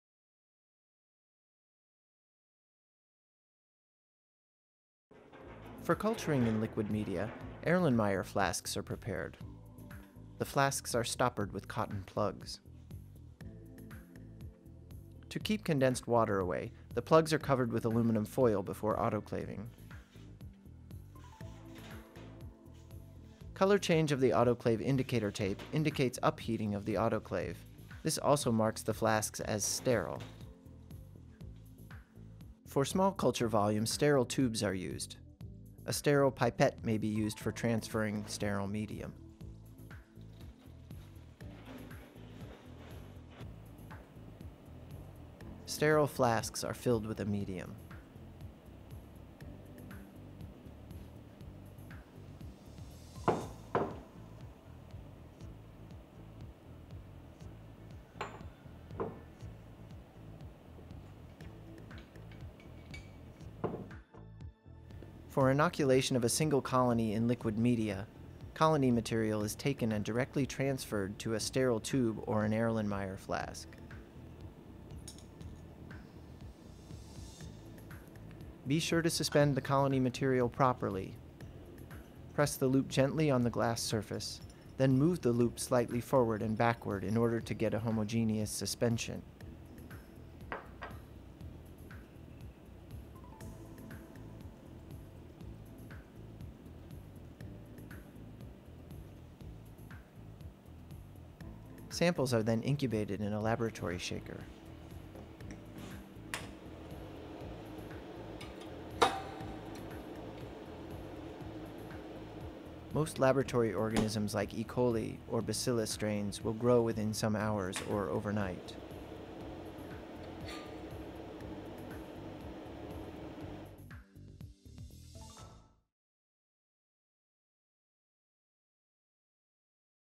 Basic Techniques in Microbiology - Liquid media Cultures
Cultures of bacteria are a basic element used in microbiolgy. This film shows the proper way of culturing in liquid media.

German version is also availabe.